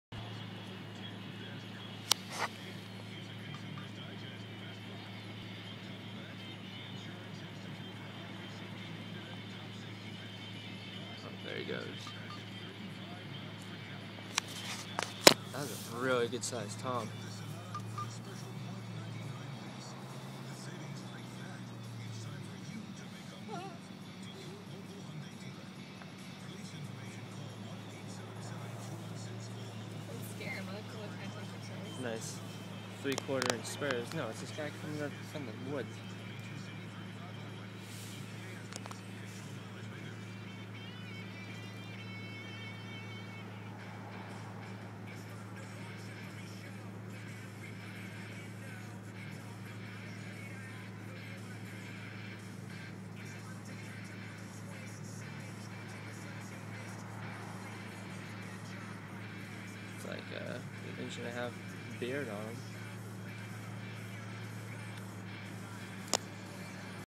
Lots of turkeys in Weymouth
Saw this flock driving around Weymouth, Ma. 1 Jake, maybe a Tom, with 6 or 7 hens.